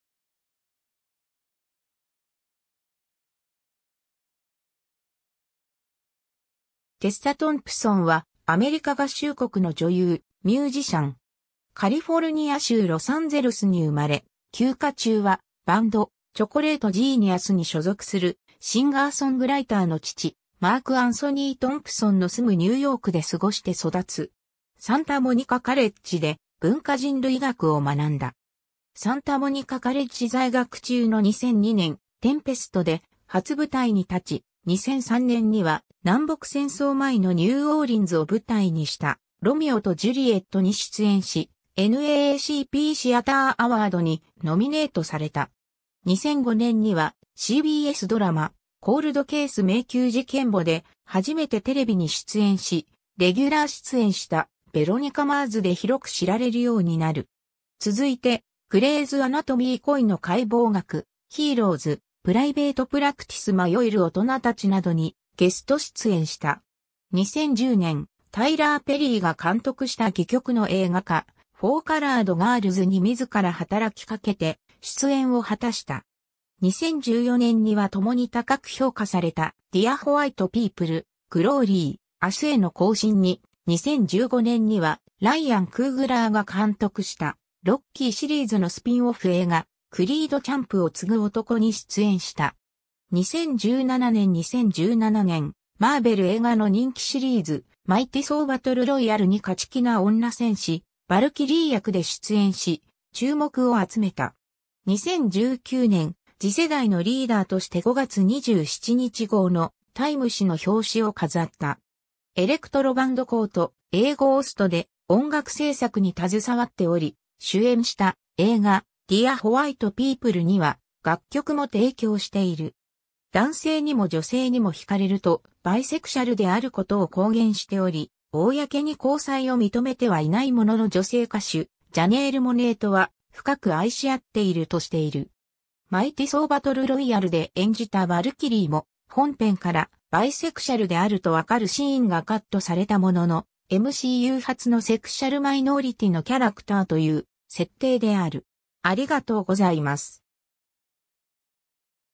テッサ・トンプソン
テッサ・トンプソン（Tessa Thompson、1983年10月3日 - ）は、アメリカ合衆国の女優、ミュージシャン。
 カリフォルニア州ロサンゼルスに生まれ、休暇中はバンド、チョコレート・ジーニアスに所属するシンガーソングライターの父マーク・アンソニー・トンプソンの住むニューヨークで過ごして育つ。
サンタモニカカレッジで文化人類学を学んだ。
 サンタモニカカレッジ在学中の2002年、『テンペスト』で初舞台に立ち、2003年には南北戦争前のニューオーリンズを舞台にした『ロミオとジュリエット』に出演しNAACPシアター・アワードにノミネートされた。
 2005年にはCBSドラマ『コールドケース 迷宮事件簿』で初めてテレビに出演し、レギュラー出演した『ヴェロニカ・マーズ』で広く知られるようになる。
続いて『グレイズ・アナトミー 恋の解剖学』『HEROES』『プライベート・プラクティス 迷えるオトナたち』などにゲスト出演した。
 2010年、タイラー・ペリーが監督した戯曲の映画化『For Colored Girls』に自ら働きかけて出演を果たした。
2014年にはともに高く評価された『ディア・ホワイト・ピープル』『グローリー/明日への行進』に、2015年にはライアン・クーグラーが監督した『ロッキー』シリーズのスピンオフ映画『クリード チャンプを継ぐ男』に出演した。
 2017年 2017年、マーベル映画の人気シリーズ『マイティ・ソー バトルロイヤル』に勝気な女戦士ヴァルキリー役で出演し、注目を集めた。
 2019年、次世代のリーダーとして5月27日号のTIME（タイム）誌の表紙を飾った。
 エレクトロ・バンドCaught a Ghostで音楽制作に携わっており、主演した映画『ディア・ホワイト・ピープル』には楽曲も提供している。
 「男性にも女性にも惹かれる」とバイセクシャルであることを公言しており、公に交際を認めてはいないものの女性歌手ジャネール・モネイとは「深く愛し合っている」としている。
『マイティ・ソー バトルロイヤル』で演じたヴァルキリーも、本編からバイセクシャルであると分かるシーンがカットされたものの、MCU初のセクシャル・マイノリティのキャラクターという設定である。